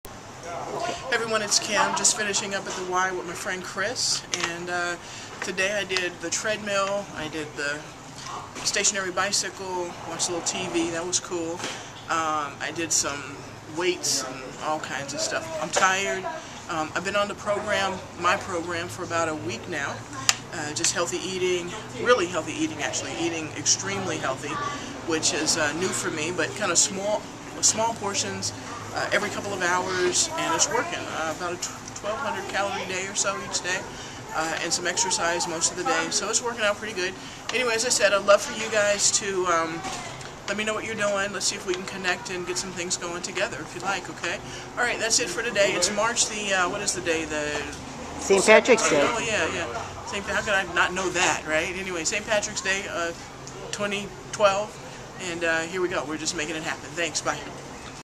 Day 7 A Bit Healthier --- March 17
I have been eating healthier and exercising daily trying to make it a lifestyle and not a diet.